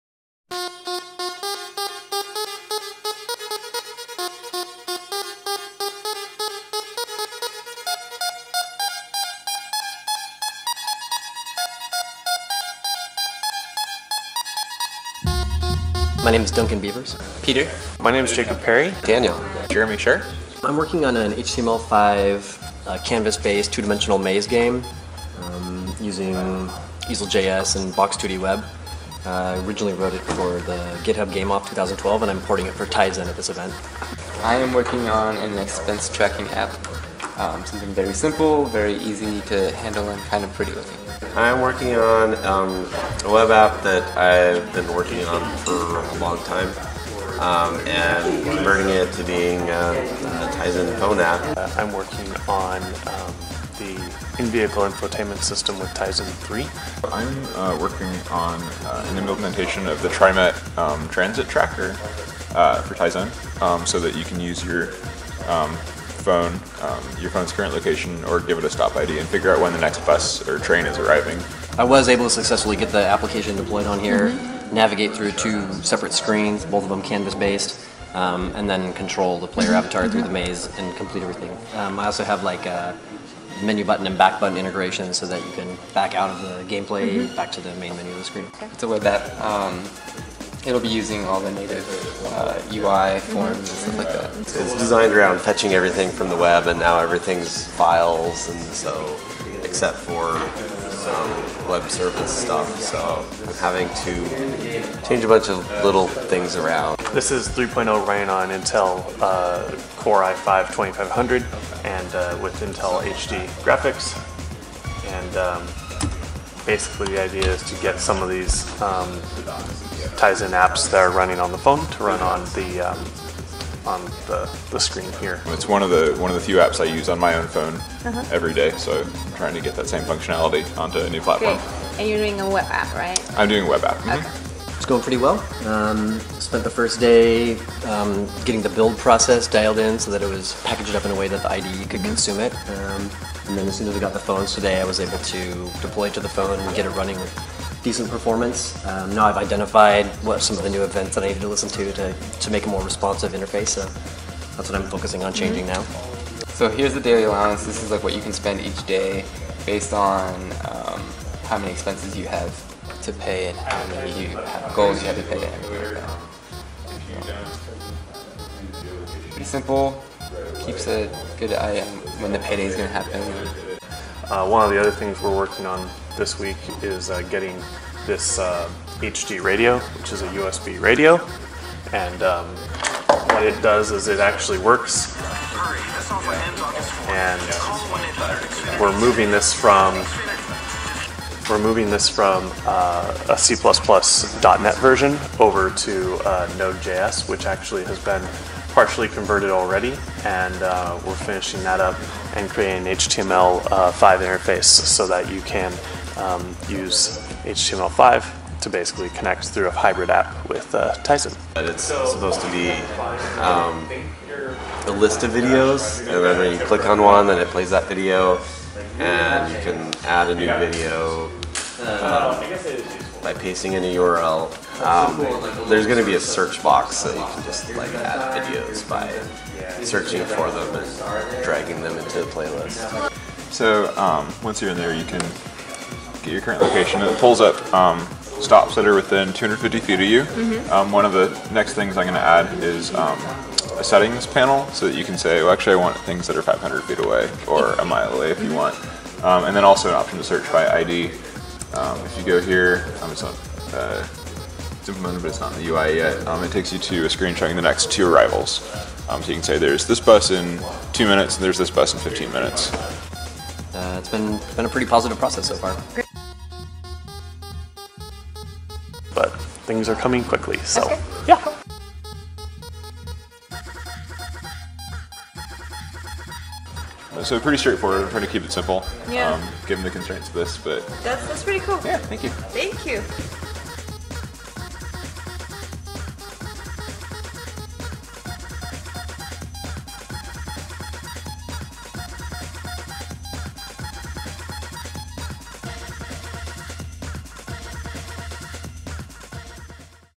Tizen Devlab & Hack Portland
BeMyApp, in cooperation with our friends at Intel and Samsung, made their Portland, Orgeon debut with the Tizen Devlab and Hack at Urban Airship. Check out our quick vid to see what the amazing developers were up to and who won prizes!